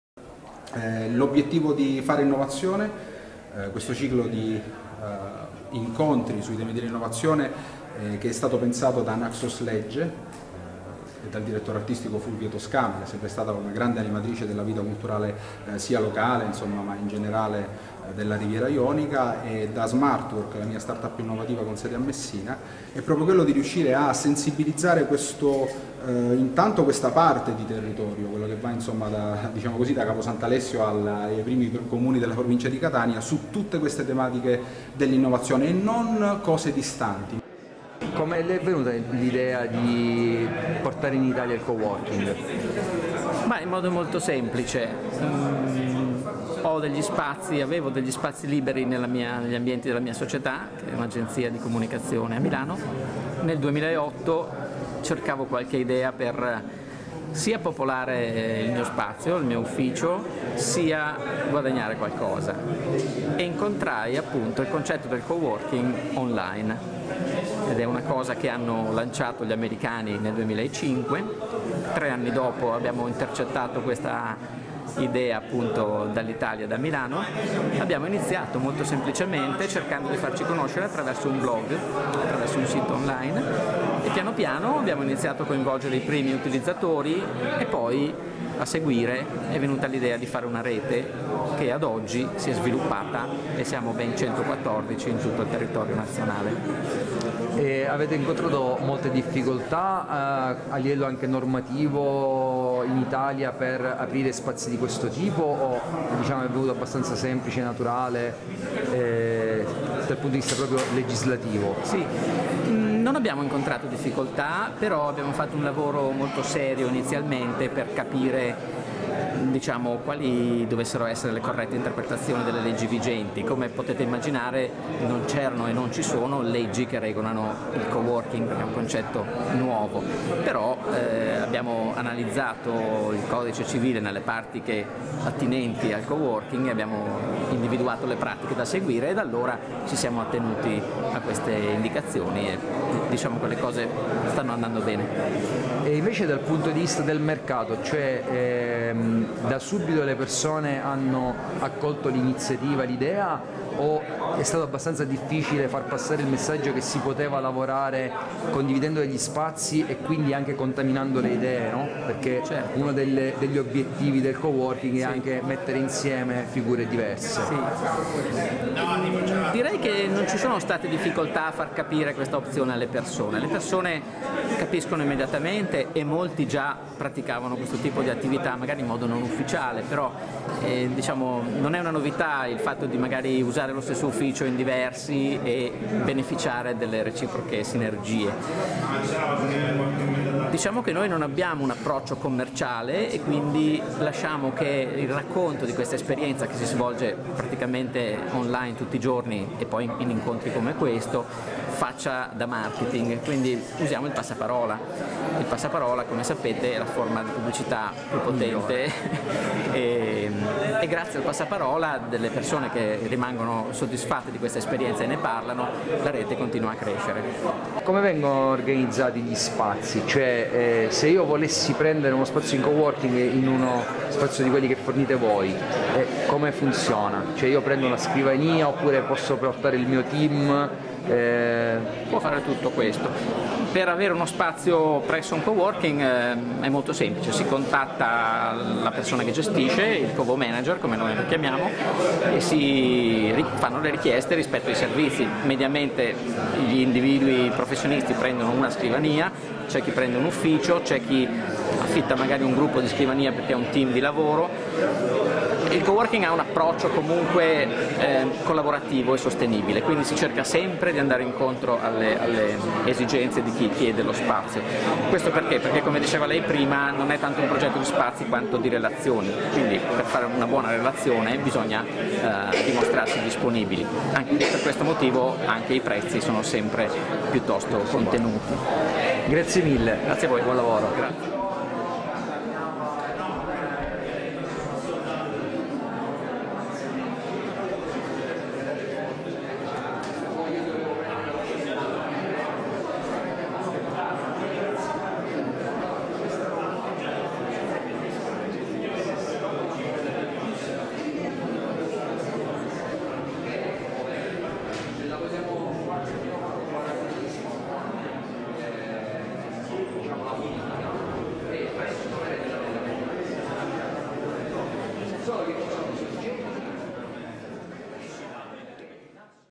Giardini Naxos, l’attualità del coworking
Si è tenuto il primo incontro sul “Fare innovazione”. Al centro del dibattito c’è stato il coworking ed è stata l’occasione per comprendere come cambia il mondo del lavoro in tutti i suoi aspetti.